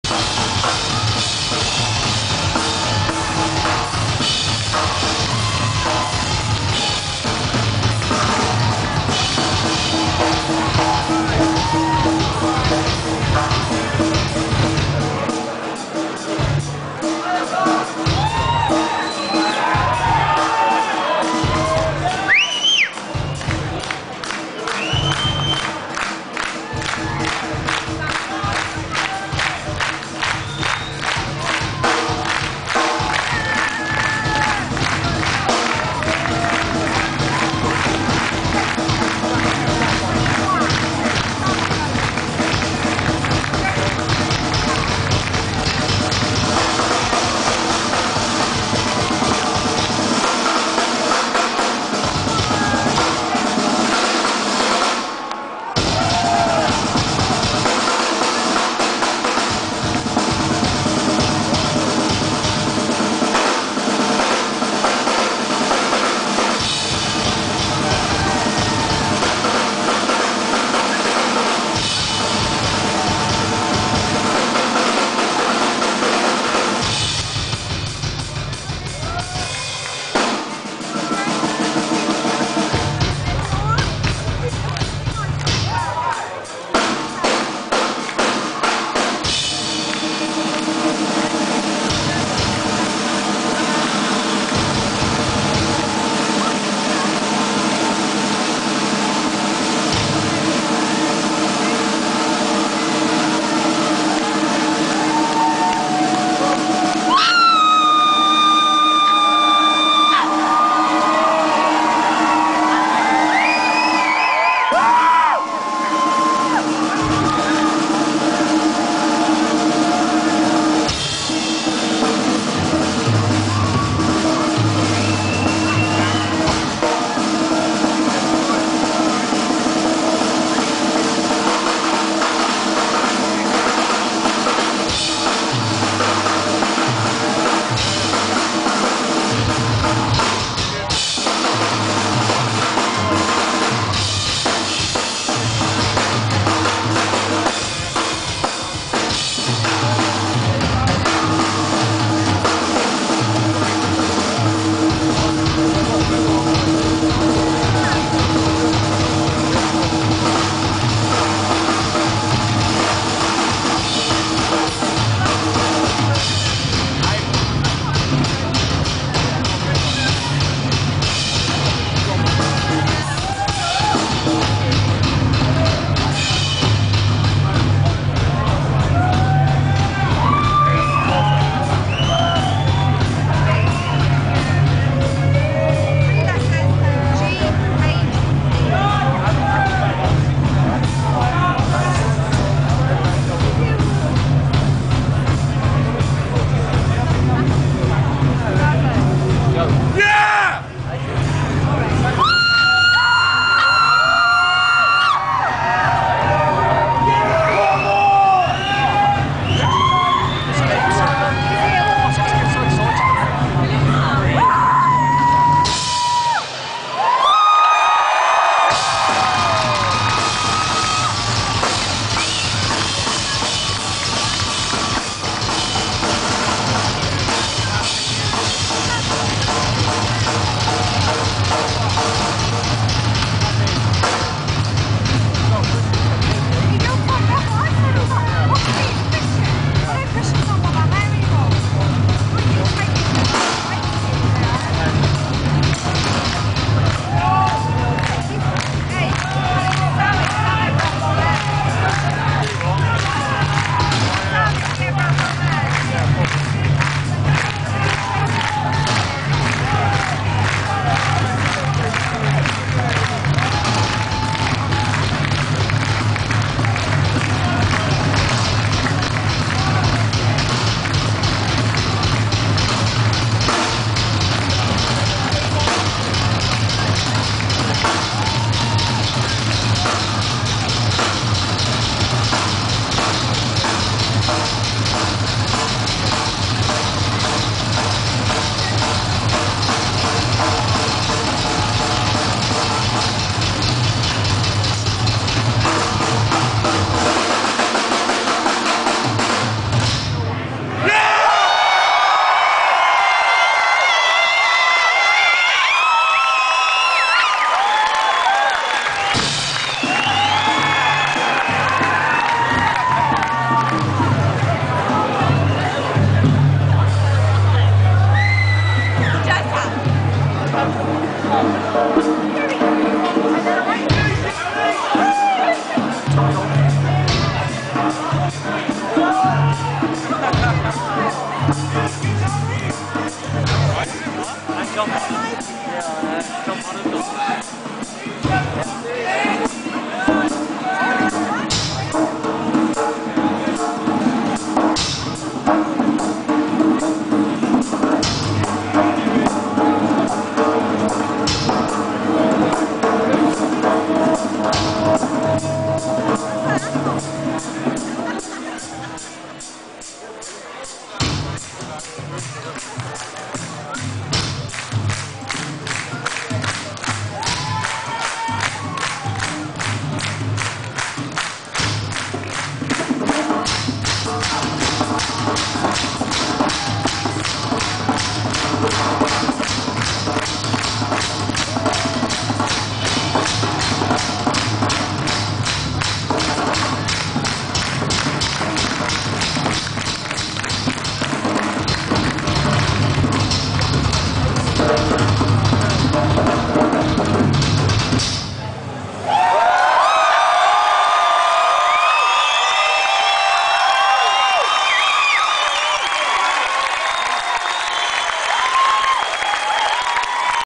Black Stone Cherry Live @ The Birmingham Academy 10-12-08
This Is The Drum Solo...Pretty Good For Such A Small Kit